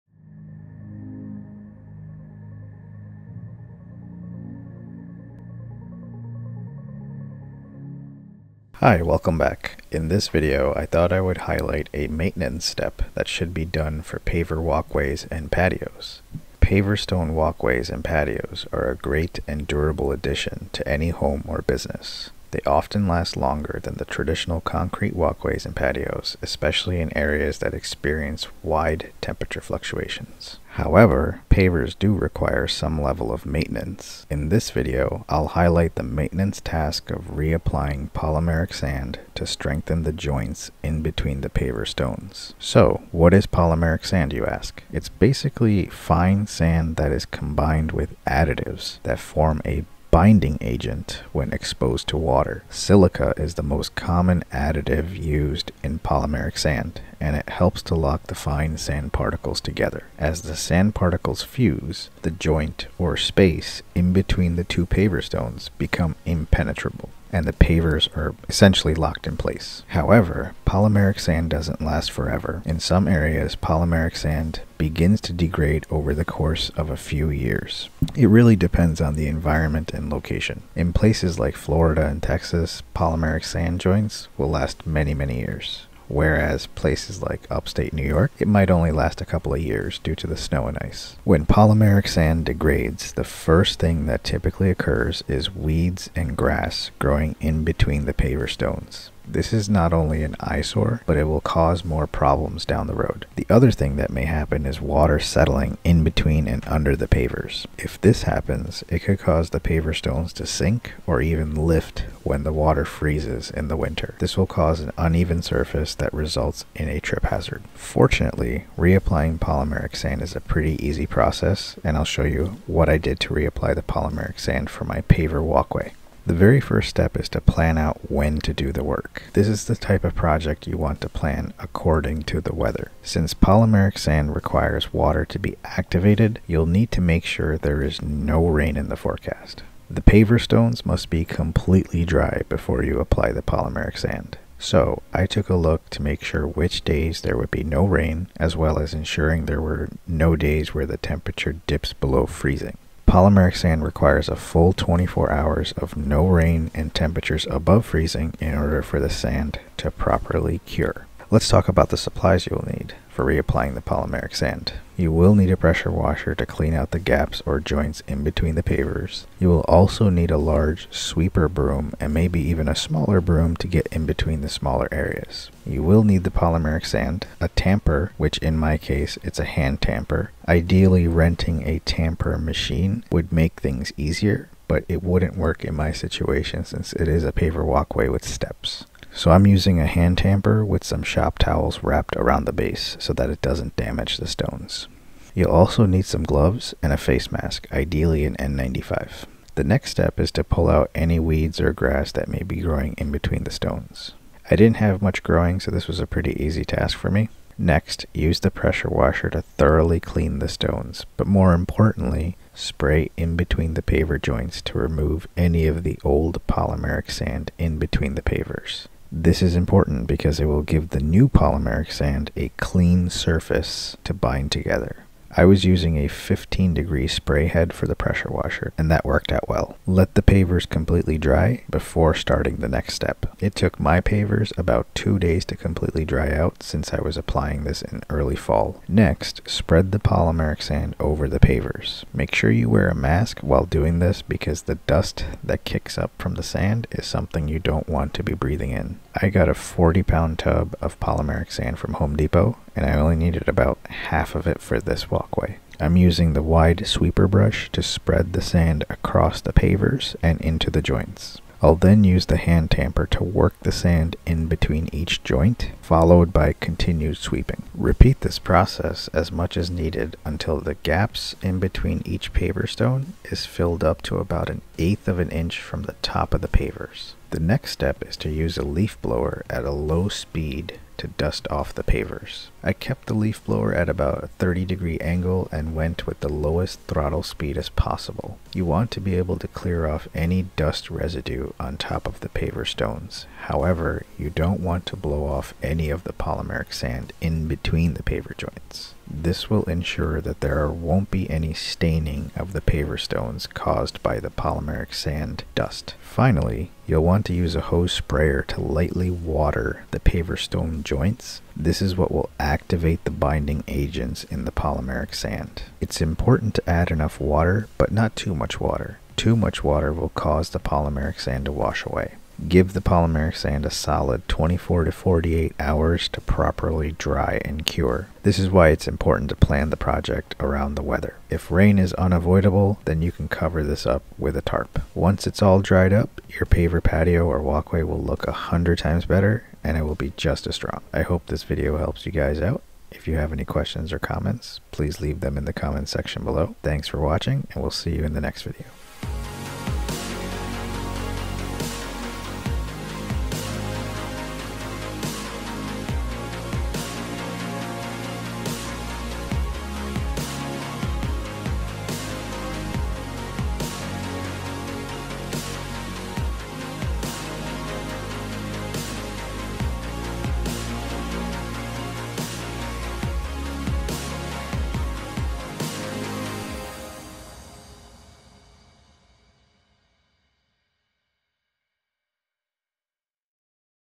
Easy Maintenance For Your Paver Walkway or Patio - Re-applying Polymeric Sand
In this video, we will guide you through the essential steps for easy maintenance of your paver walkway or patio by re-applying polymeric sand. Keeping your pavers in top condition not only enhances the beauty of your outdoor space but also ensures their durability and longevity. Learn the best paver cleaning techniques and discover effective paver maintenance tips that will help you maintain your hardscape solutions effortlessly.

We’ll cover everything you need to know about polymeric sand, including how to apply it correctly for optimal results. Whether you’re looking to rejuvenate your existing paving stones or considering sustainable hardscaping solutions, this video provides practical insights into paver upkeep. From paver cleaning methods to washing techniques, we will equip you with all the knowledge necessary to keep your garden pathways and patios looking fresh and inviting.

Join us as we explore the world of paver maintenance hacks and the benefits of using polymeric grout for enhanced stability. Don’t miss out on these valuable tips to ensure your pavers remain not only beautiful but also functional for years to come. Watch now to learn how to achieve the best results in your paver maintenance journey!

Chapters:
0:09 - Intro
0:31 - Why it's important to maintain paver walkways and patios
0:44 - What is polymeric sand
1:11 - What happens to polymeric sand over time
1:31 - Why it's important to re-apply polymeric sand
2:05 - Process to re-apply polymeric sand
5:40 - Outro

-Wide sweeping broom
-Regular broom
-Polymeric sand (40lb) from Home Depot
-Hand tamper